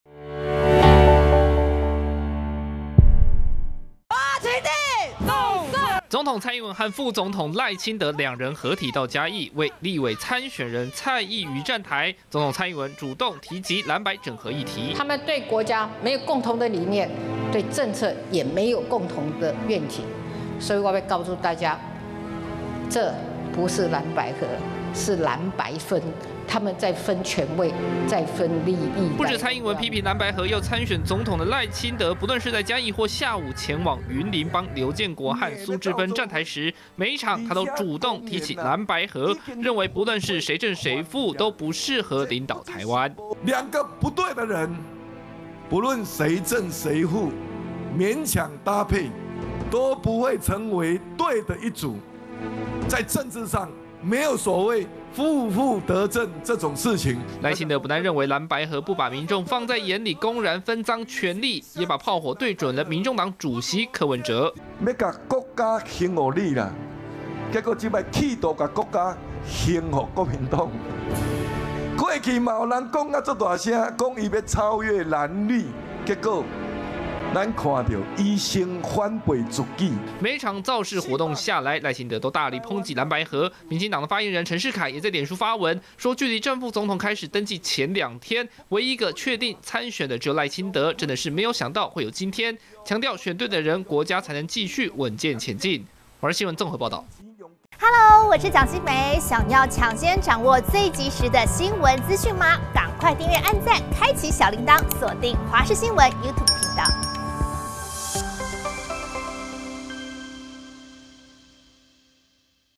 這不是藍白合！ 蔡英文：是藍白「分」.分權位分利益｜華視新聞 20231118
總統蔡英文上午和民進黨總統參選人賴清德兩人合體，到嘉義縣出席立委蔡易餘聯合競選總部成立大會，談到藍白合議題，蔡英文說，兩個政黨沒有共同理念，政策也沒有共同願景，不是藍白合，是藍白分，賴清德則不論是嘉義，或下午前往雲林站台時，都說：藍白合不論誰正誰負，兩個不對的人在一起，都不會負負得正，也暗諷民眾黨主席柯文哲，過去講要把國家還給人民，現在卻是企圖把國家還給國民黨。